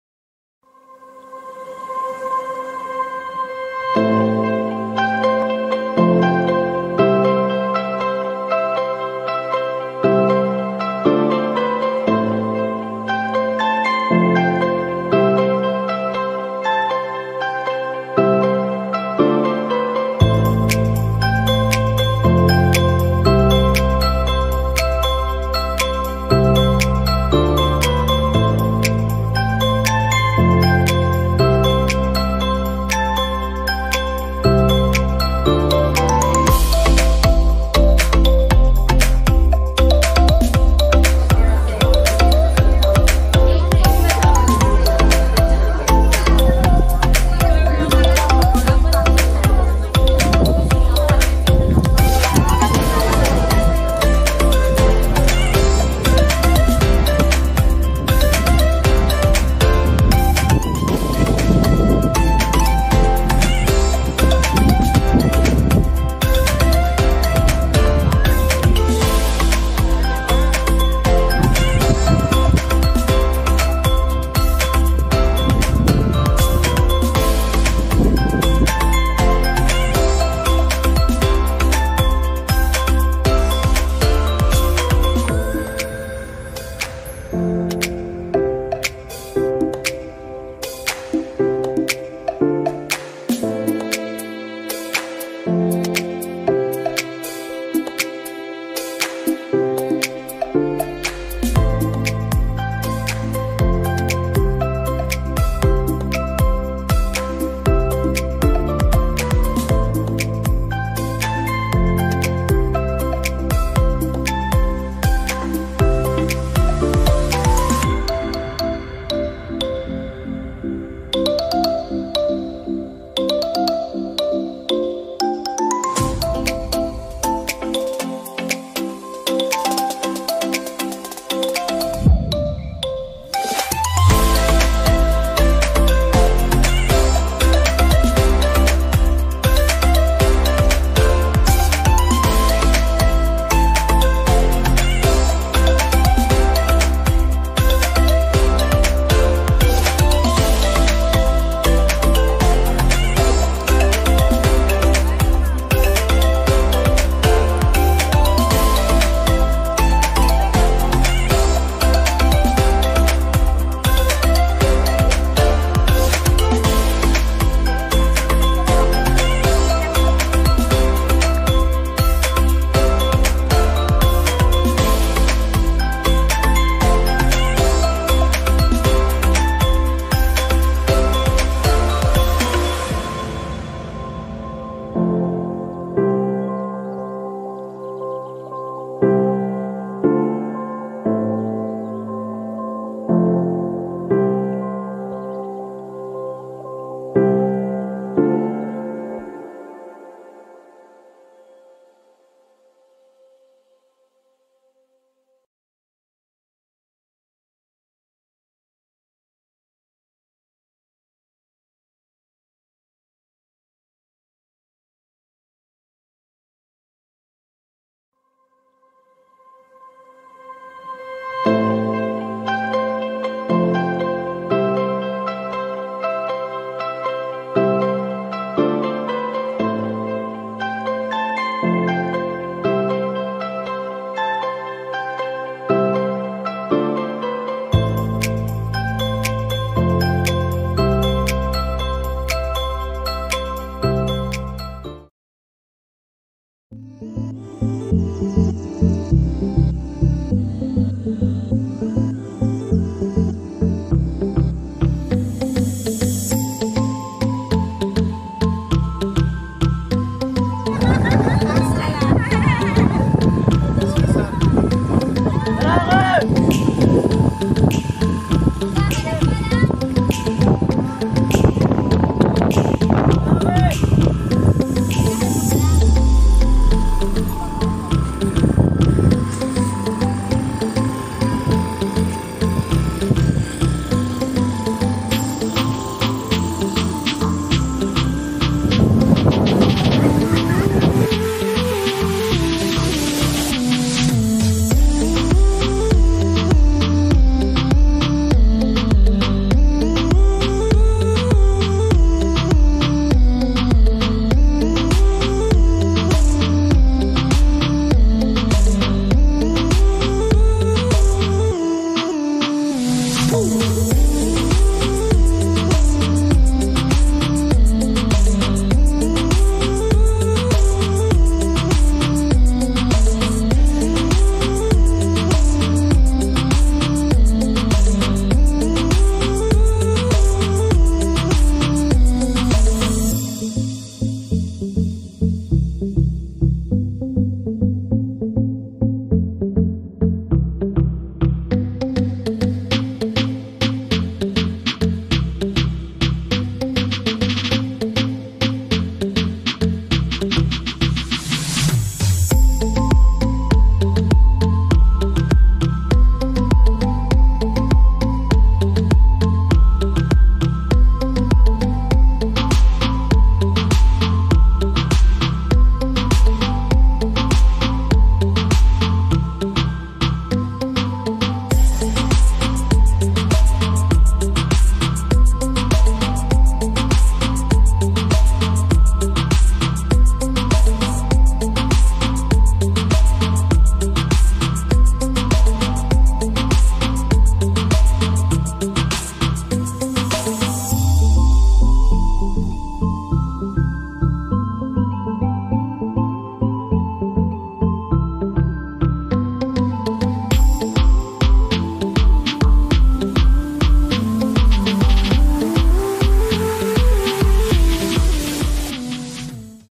Treasure Mountain + El Patio Razon
Nestled in Tanay, Rizal, Treasure Mountain is a campsite overlooking the Sierra Madre mountain range.